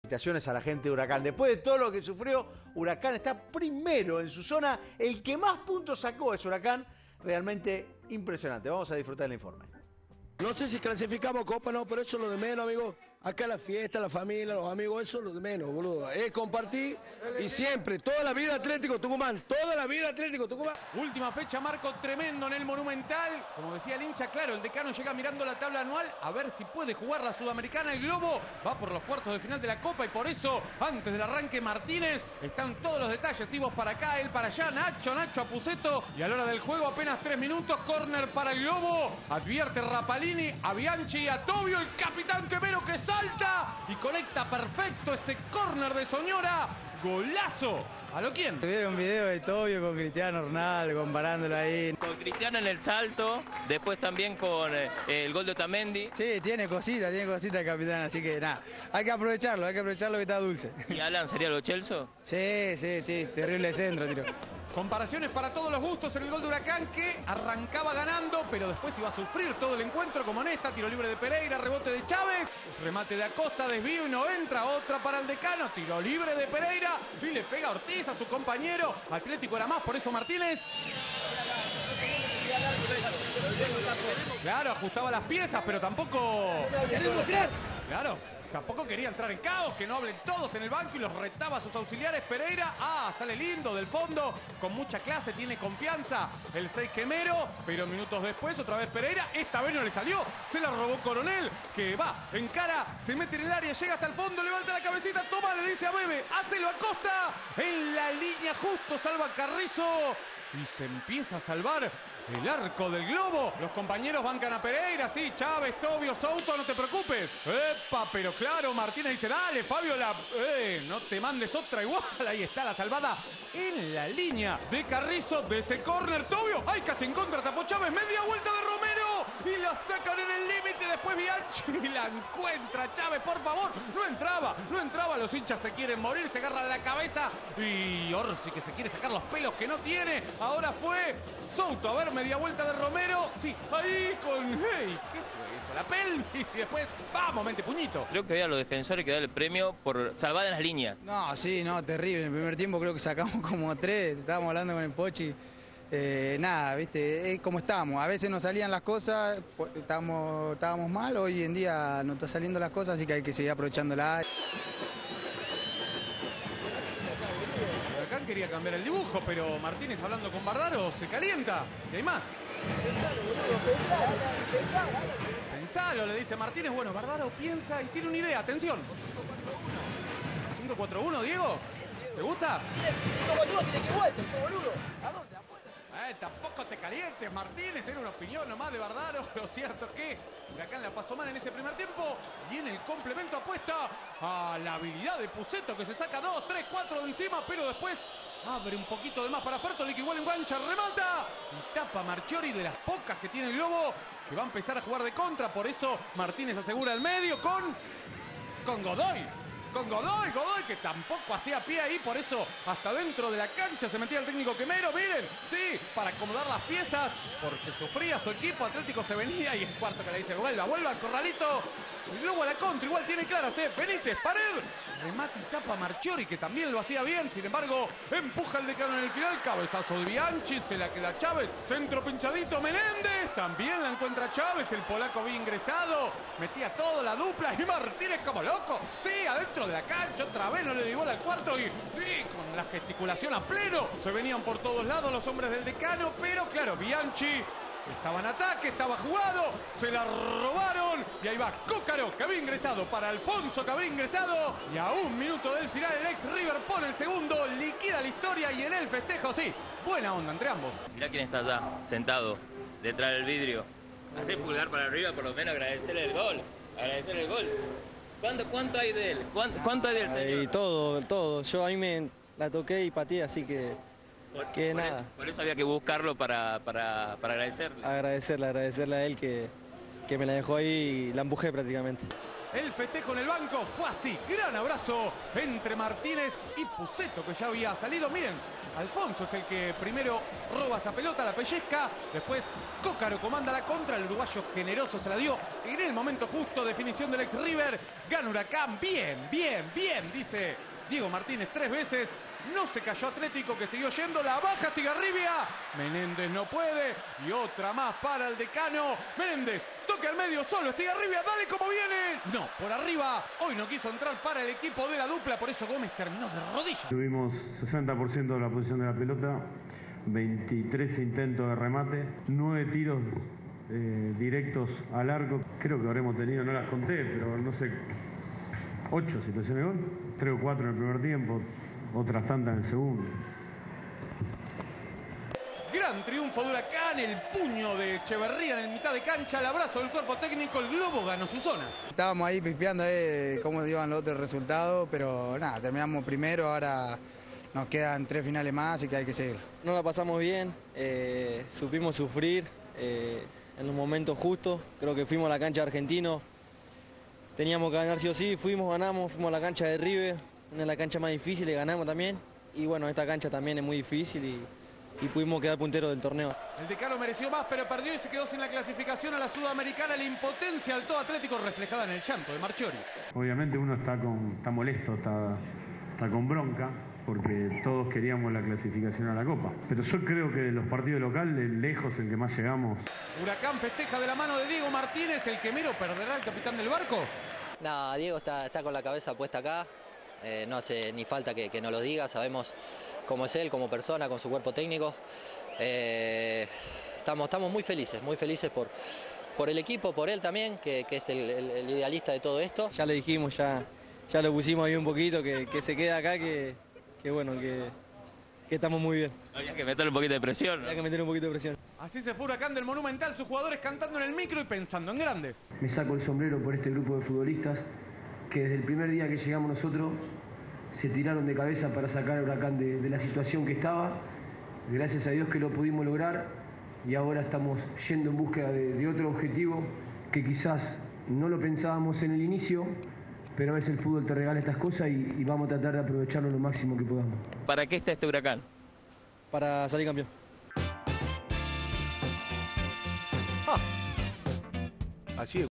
Resumen Atlético Tucumán 0 vs Huracan 2 - Fecha 14 Copa de La Liga 2023
Les dejo el resumen del triunfo de Huracan en Tucumán.